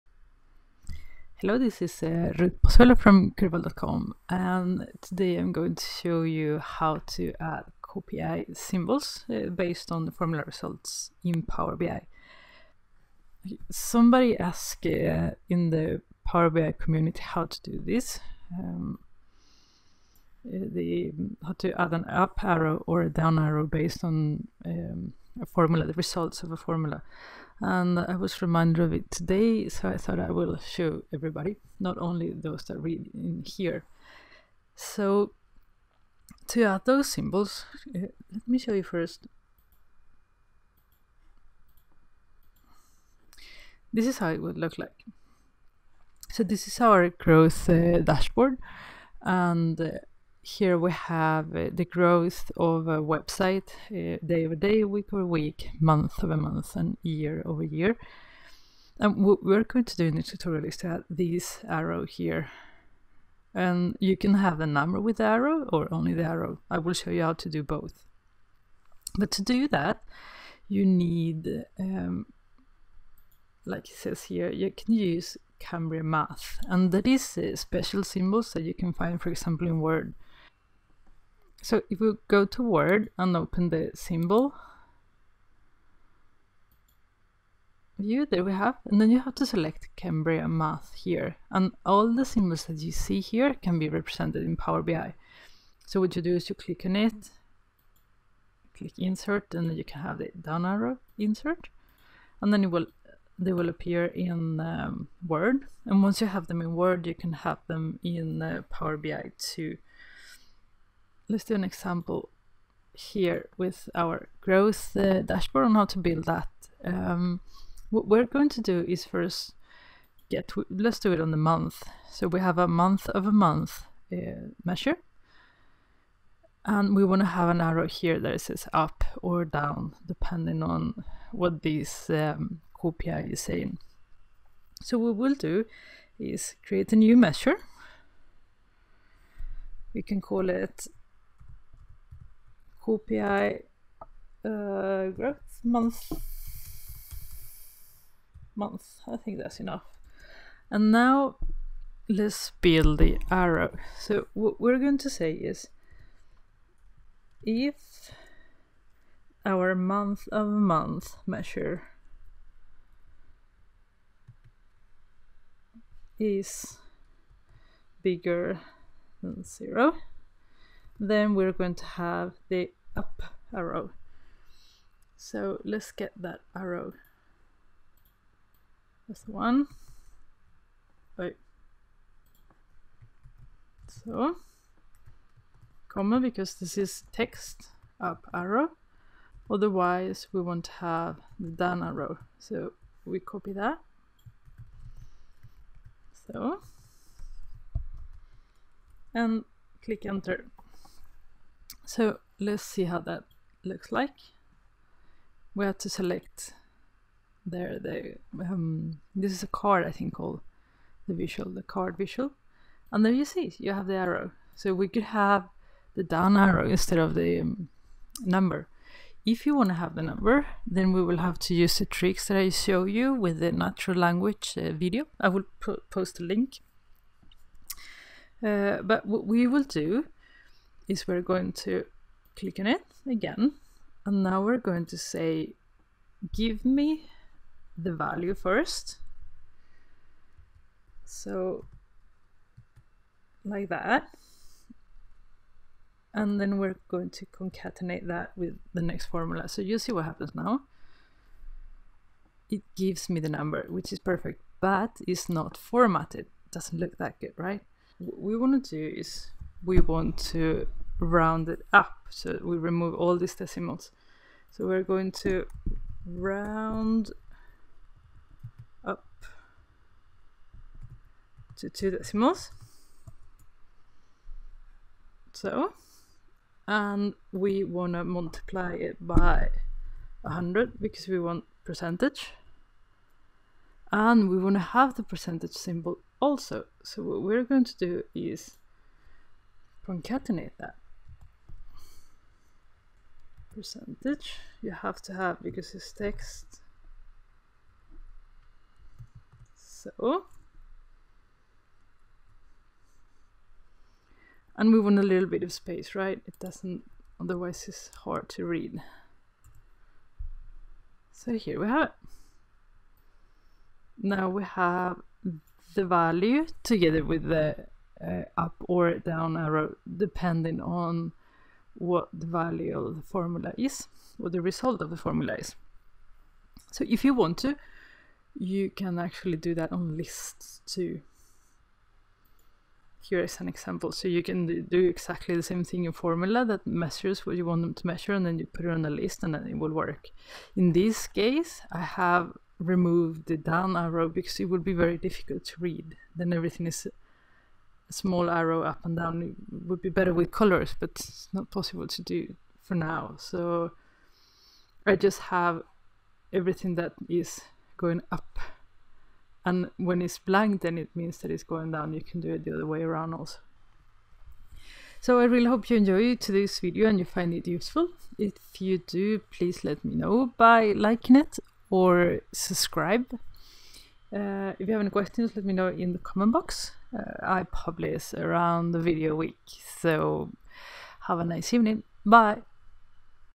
Add KPI symbols in Power BI based on formula results using DAX
Add up and down arrows to your calculations based on the results of your formulas in Power BI.